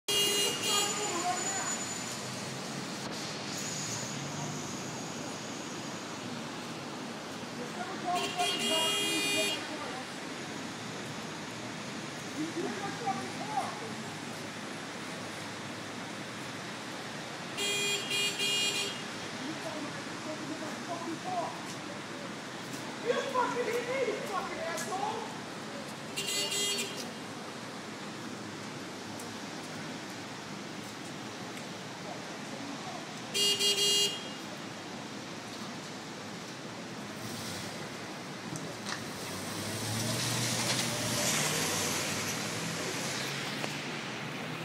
Hilarious Car Accident Rant
Guy rants on driver for bumping into car. Hitter drives away. Funny